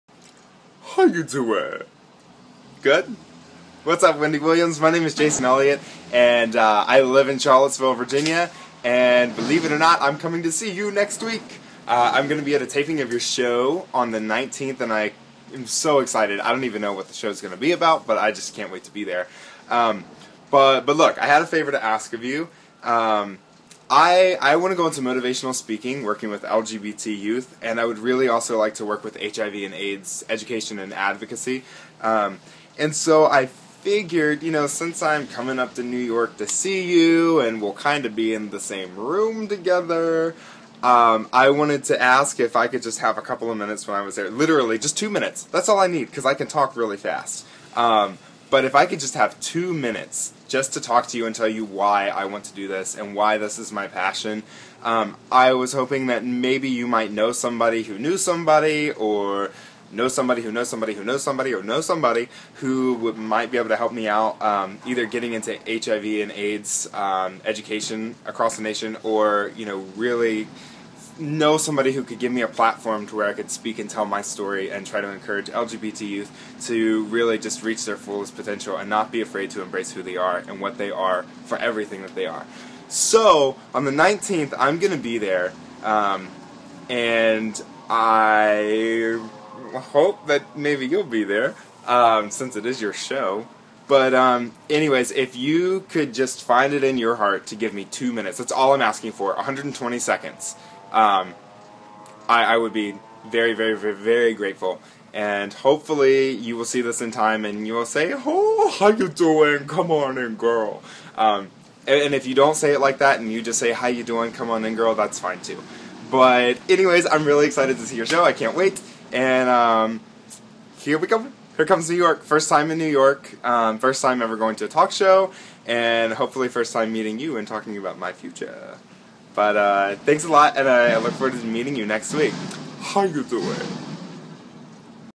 Wendy Williams! I am coming to your show!!
How you doin?! I'm doing great, but will be doing even better when I am at the Wendy Williams show next week!!

Wendy, the video says it all! I need help getting into my career and if I can have 120seconds of your day, I would be sssooooo thankful!!

I'll be at your show on June 19, 2012 and look forward to chatting with you!!